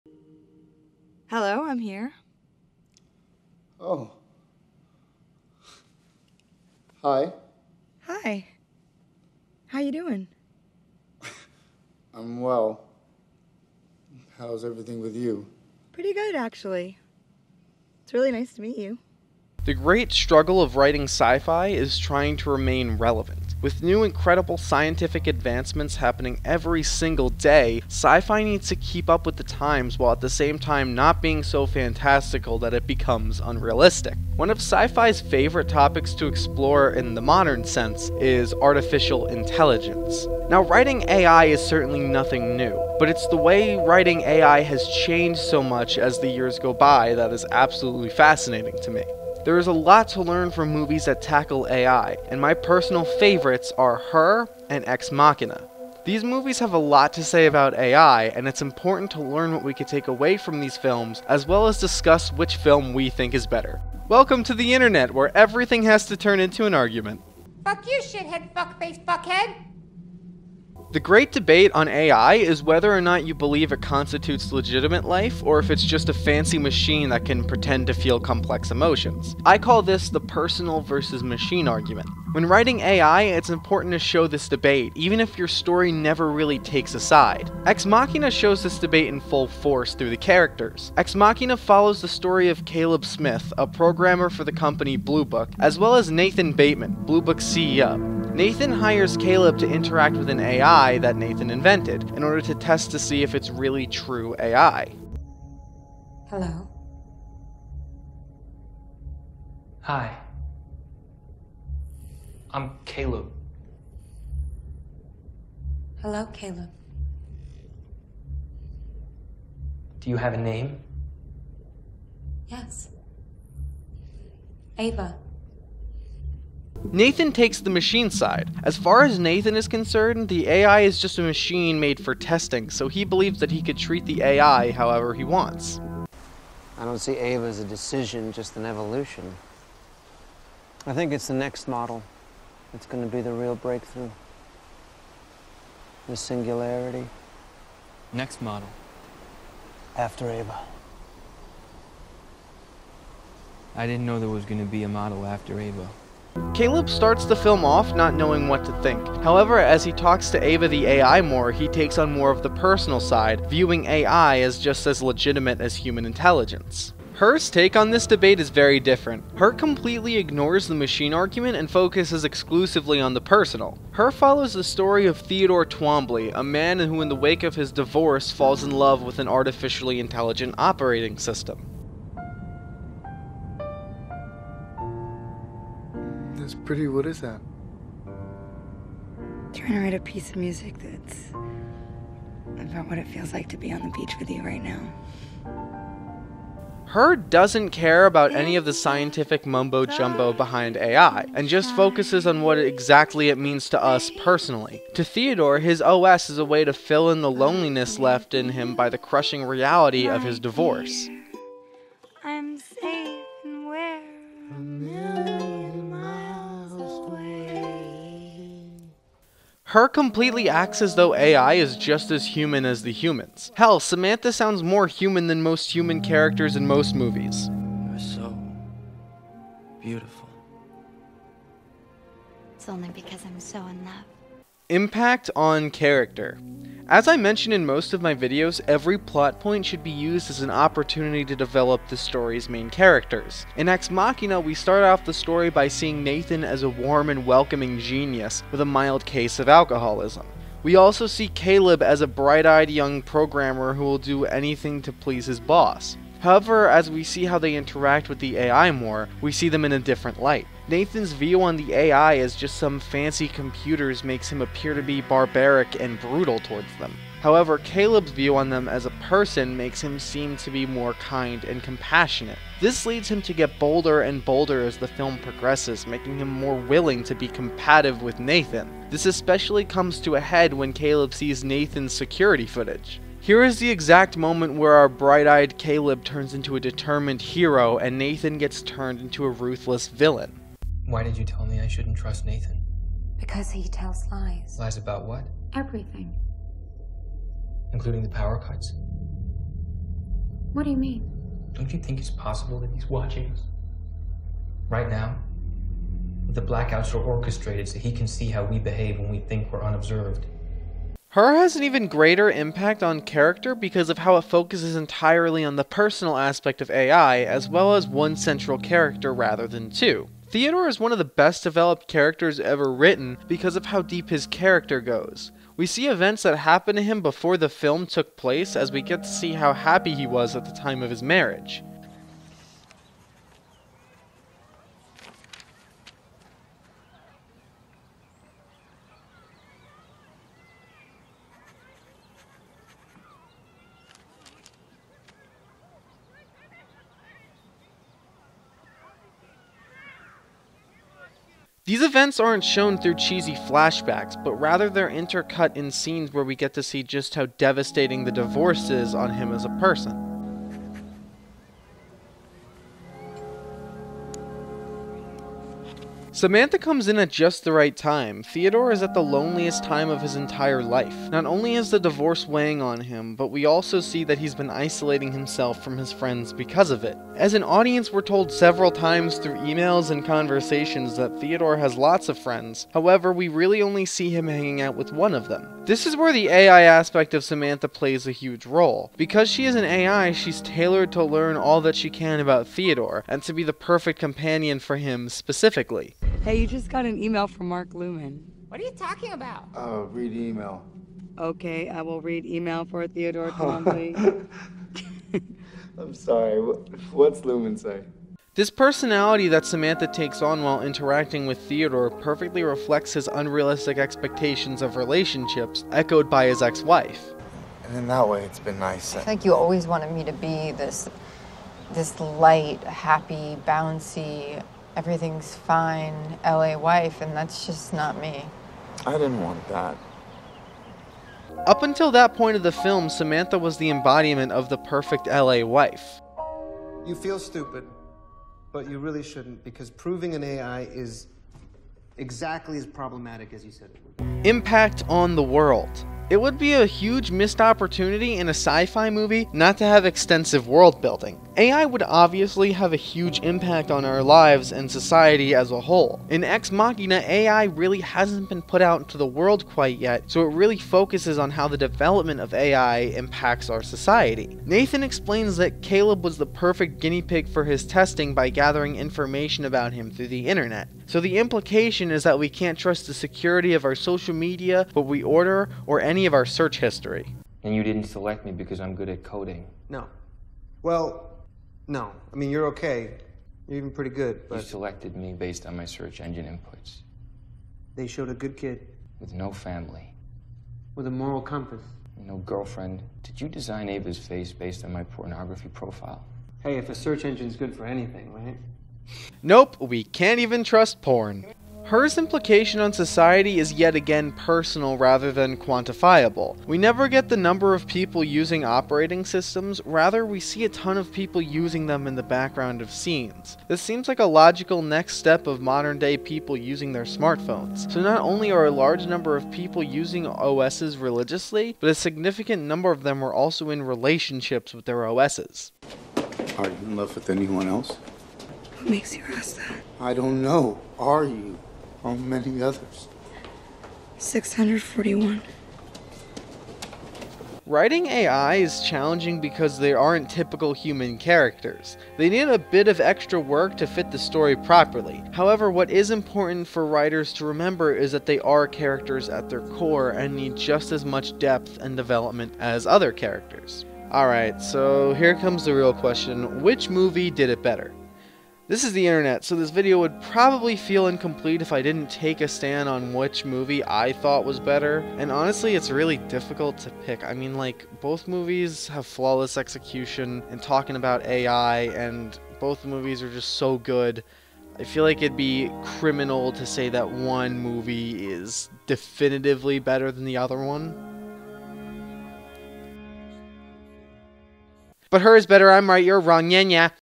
Her Vs. Ex-Machina: Writing Artificial Intelligence
In this video I discuss how you can better impliment writing Artificial Intellegence in your Science Fiction story.

My Other Books: